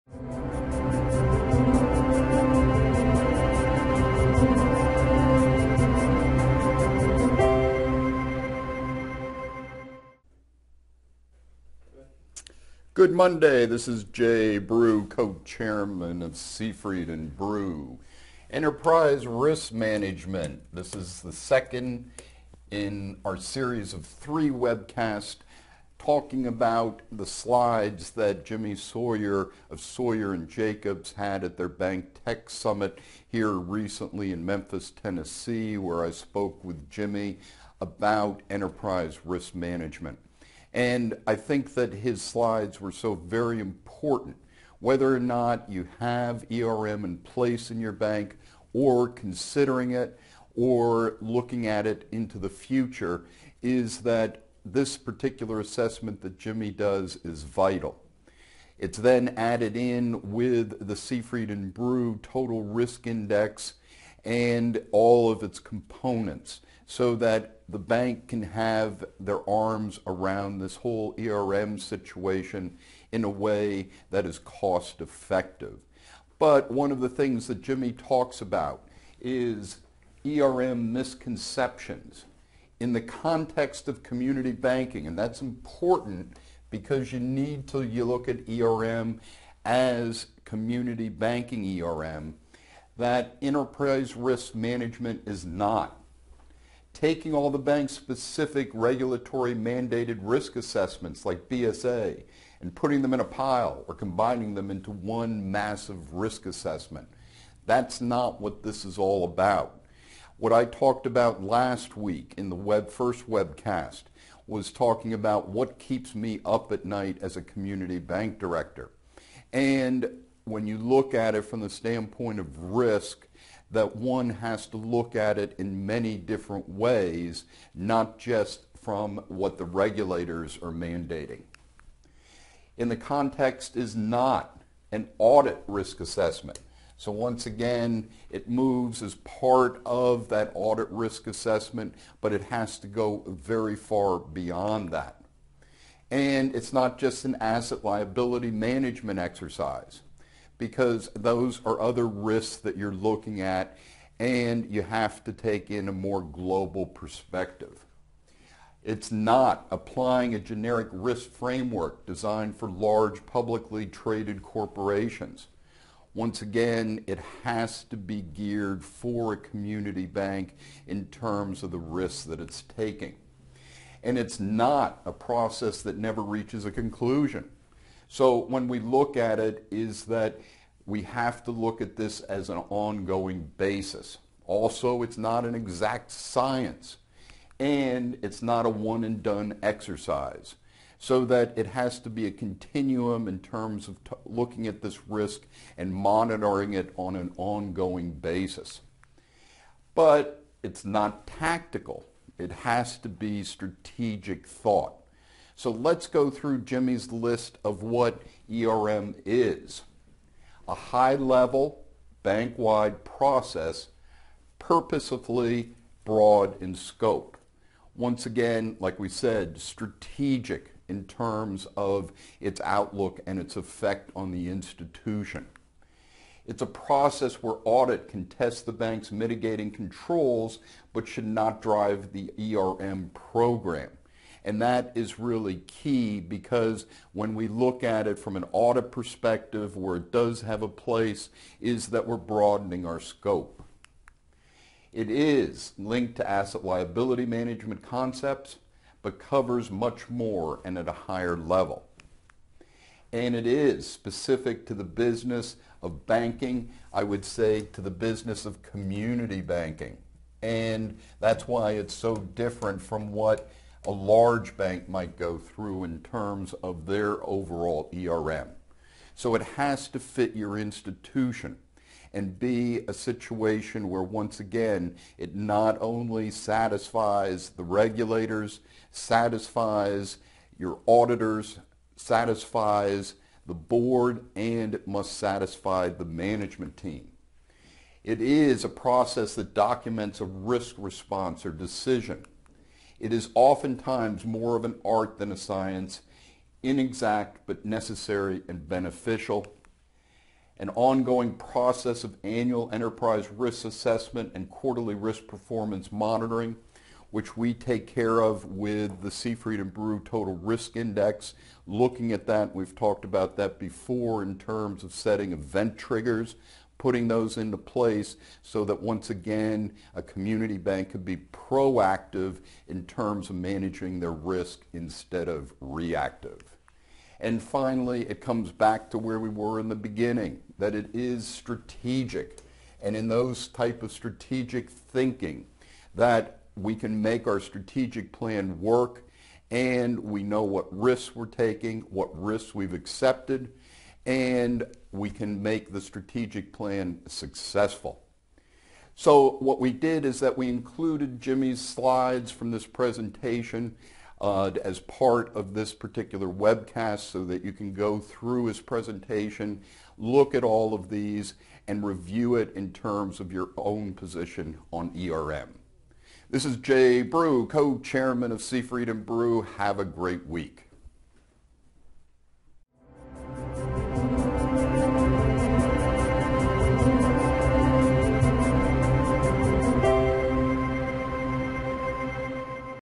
What Keeps us Up at Night - Risk (ERM Series - Part 2)
In Part 2 of our series on Enterprise Risk Management, Jay shares key considerations regarding what Enterprise Risk Management is and is not.